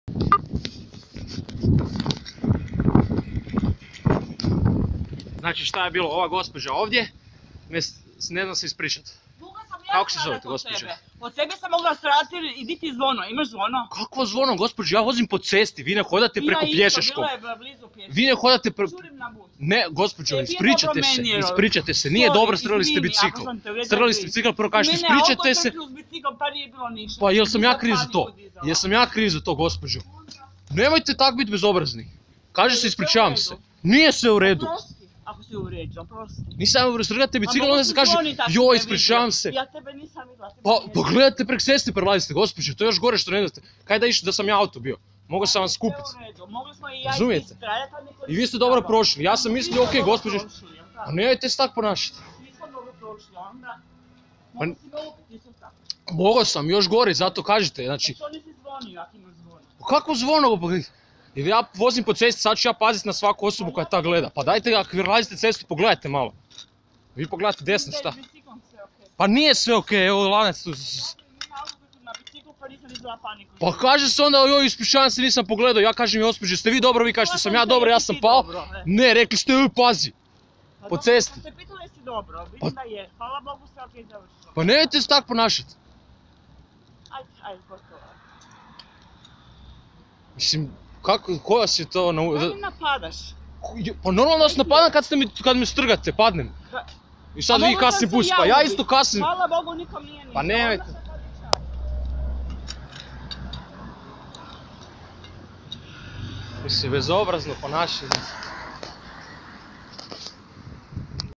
Zena bjezi s mjesta nesrece (proctaj opis)
Zena prelazi cestu usred križanja, pogleda desno vidi auto kako ide, uopce ne pogleda lijevo nege krene pretrcavat direkt ispred mene. Nakon toga ja padnem, a ona samo kaze zurim na bus i odlazi.

29.12.